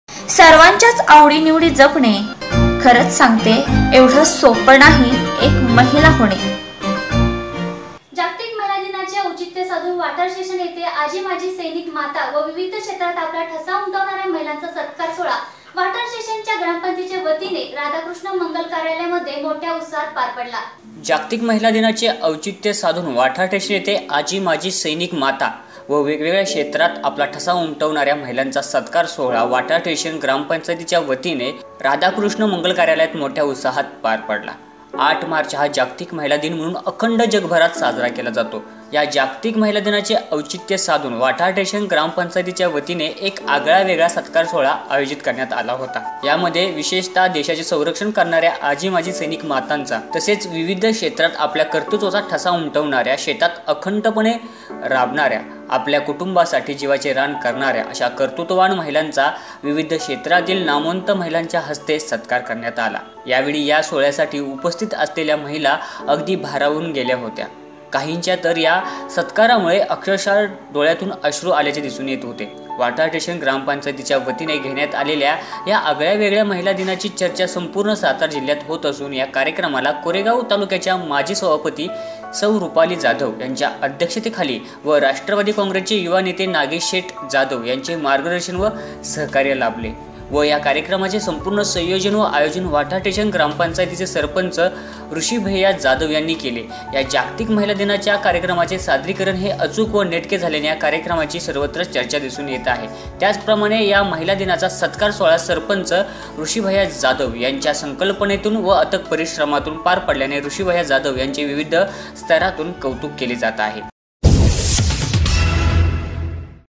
वाठार स्टेशन ग्रामपंचायतीच्या वतीने आजी माजी सैनिक माता व कर्तृत्वान महिलांचा सत्कार.
वाठार स्टेशन ग्रामपंचायतीच्या वतीने आजी माजी सैनिक माता व कर्तृत्वान महिलांचा सत्कार...पहा..फक्त...VM News Marathi